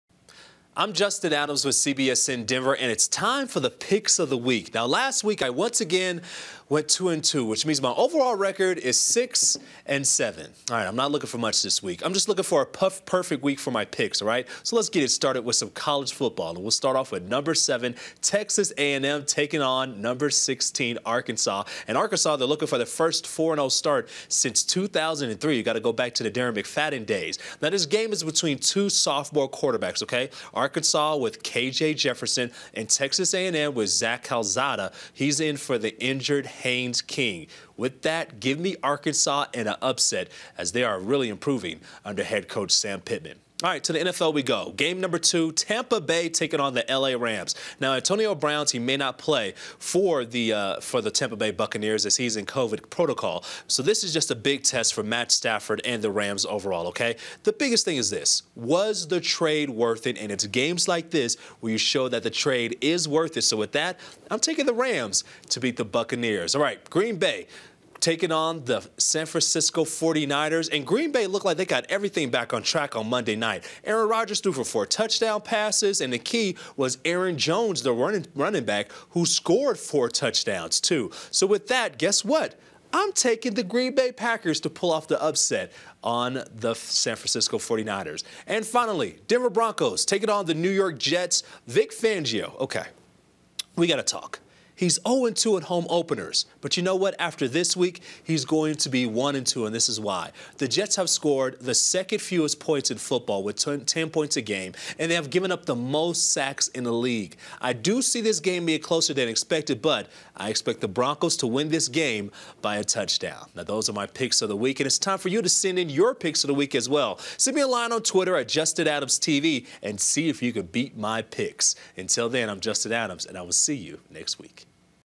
Justin’s Picks Of The Week
Justin Adams gives his picks of the top 4 games this week.